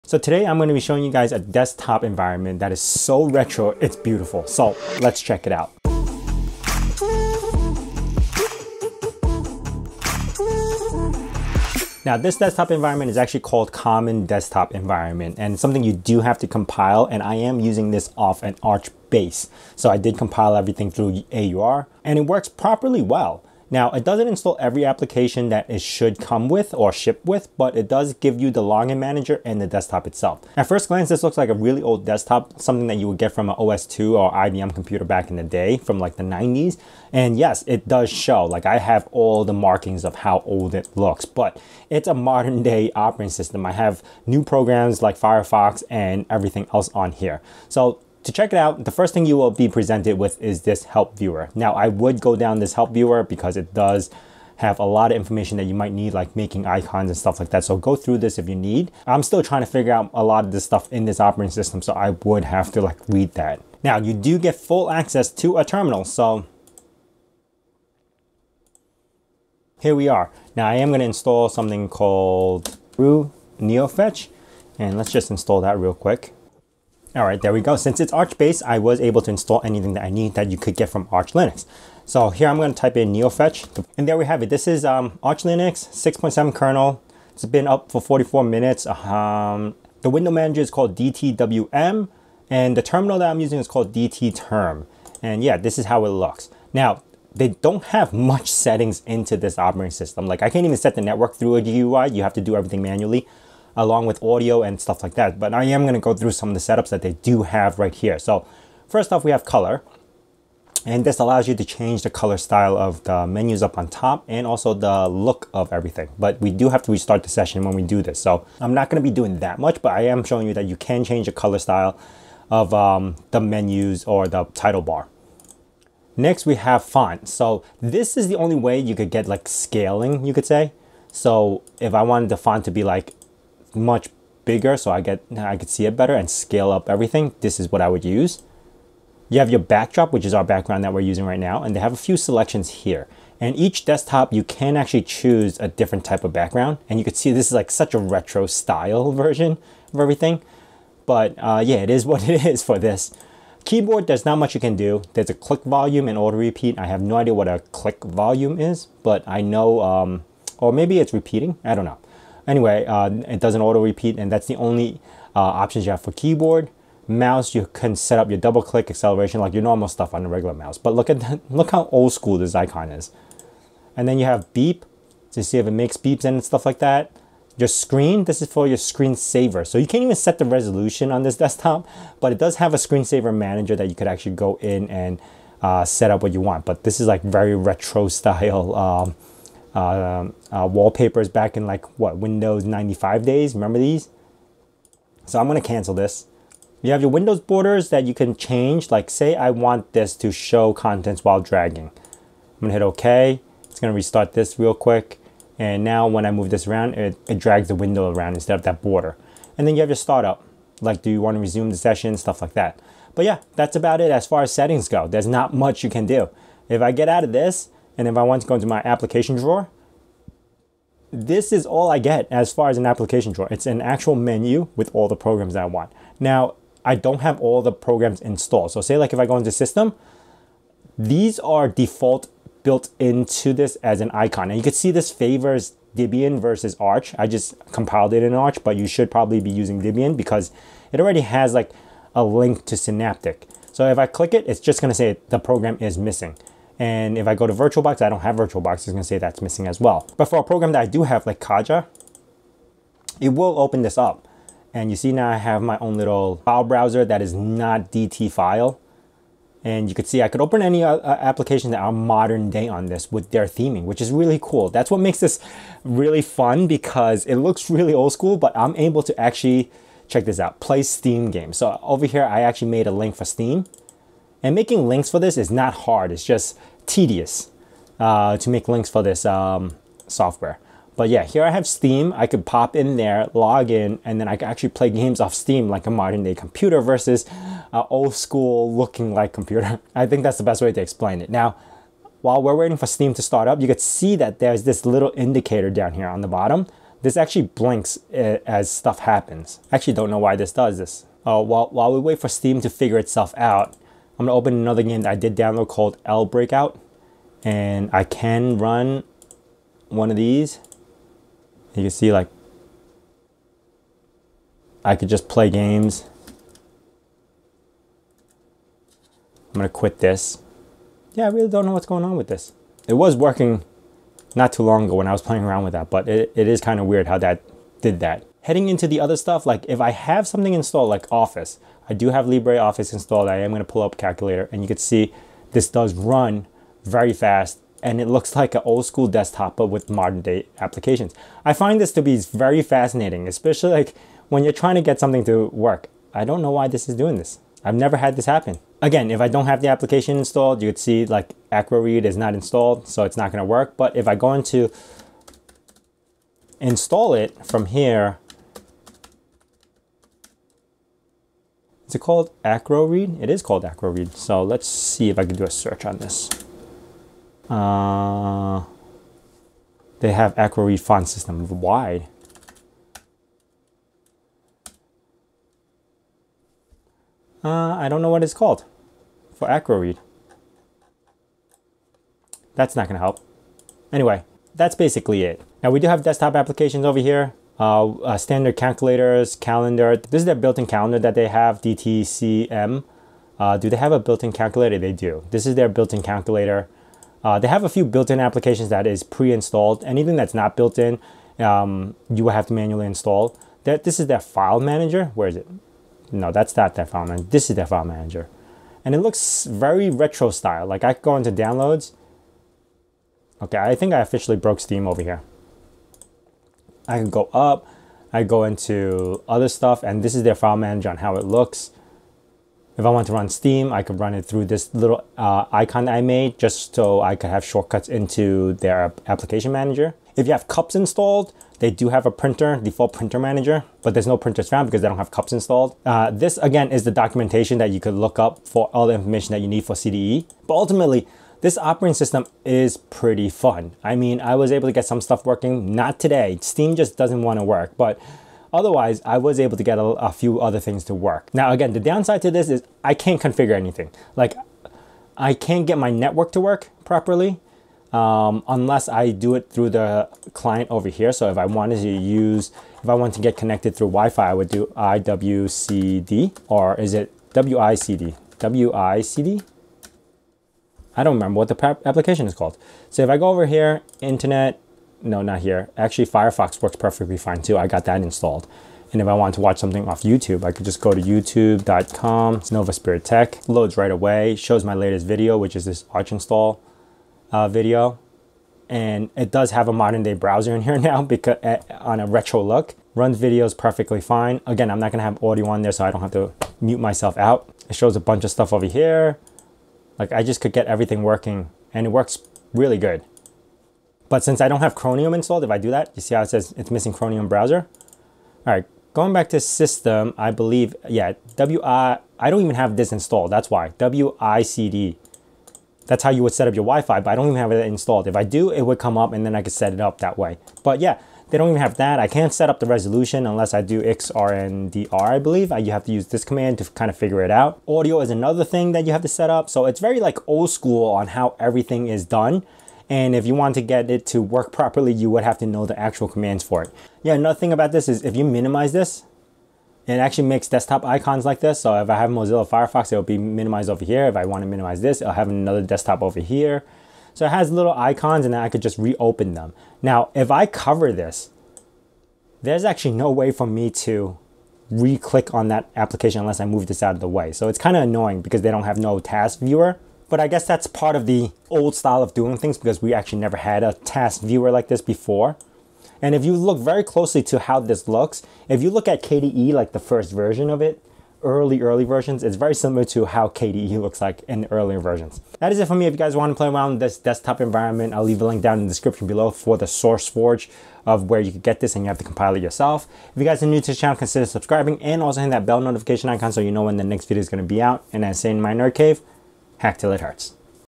This Retro Look Is So Good!
Checking out this retro desktop environment called "Common Desktop Environment" on Arch Linux. thanks for the view

○○○ LINKS ○○○

○○○ SHOP ○○○